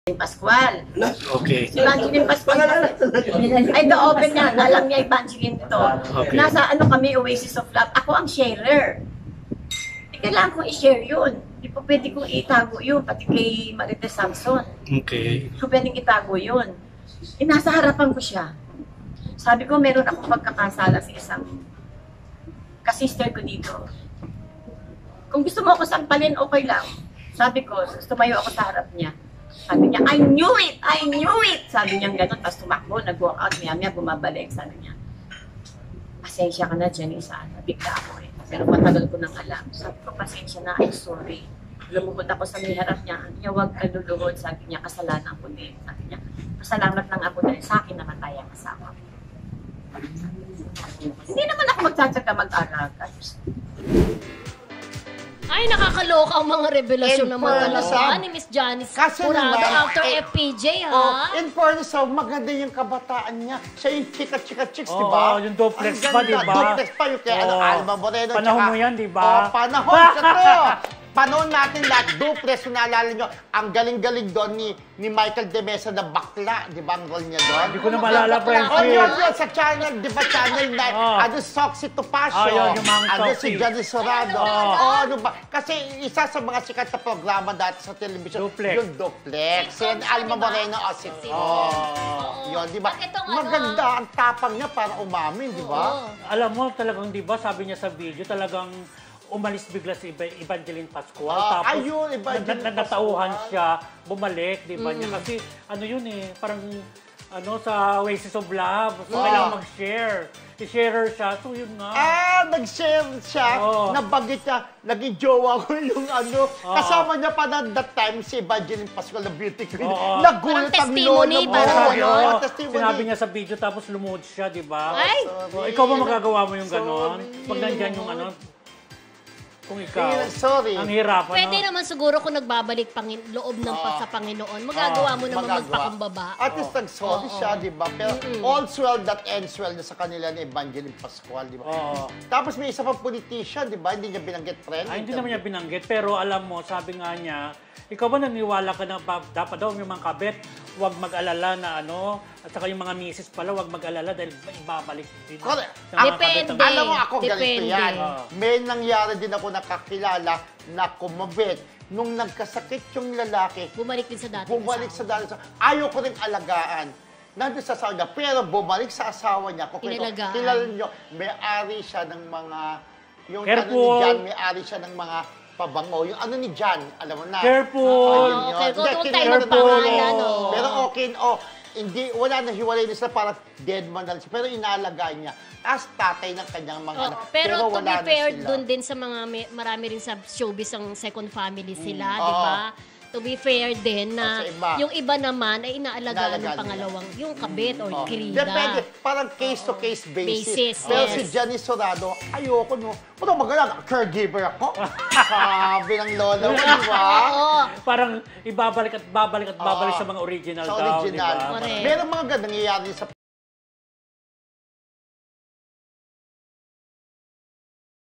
Janice inaming nakarelasyon asawa ni Evangeline Pascual | MARISOL ACADEMY QUICKIE
Nagbigay ng rebelasyon si Janice Jurado sa kanyang napagdaanan noon kung saan sinabi nito na nakarelasyon niya ang asawa ni Evangeline Pascual.